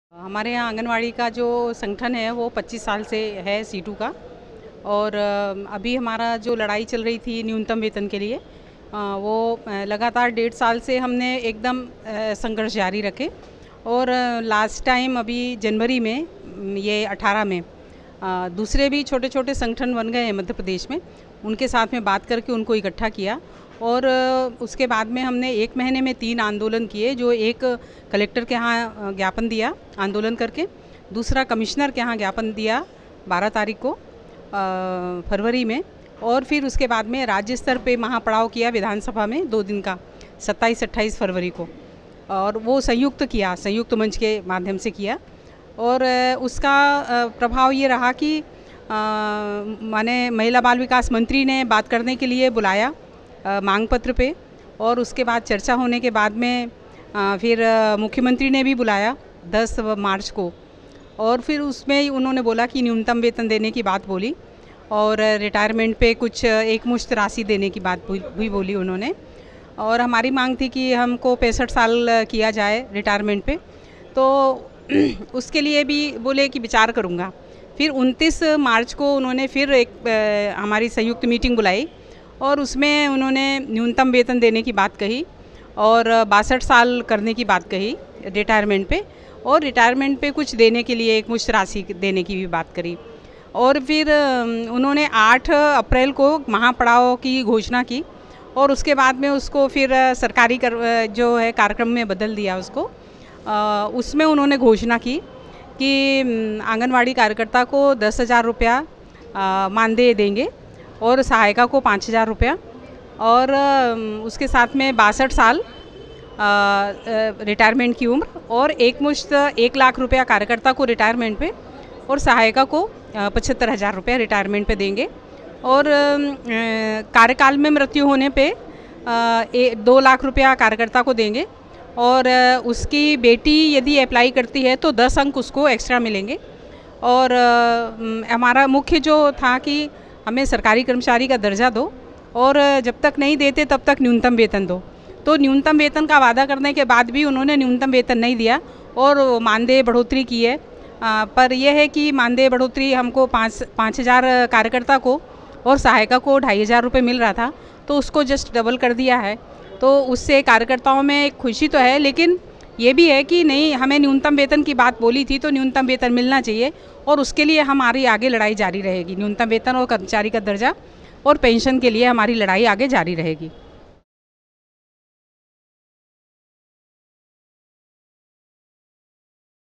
Angandwadi Workers Will Continue Fight for Their Rights Kishori Varma
A veteran of the anganwadi workers' movement in Madhya Pradesh, Comrade Kishori Varma talks about the inspiring process of organising that has seen the workers raise vital demands on wages, retirement age, pension and permanent jobs. In the face of a government reluctant to concede their demands, their defiant struggle continues.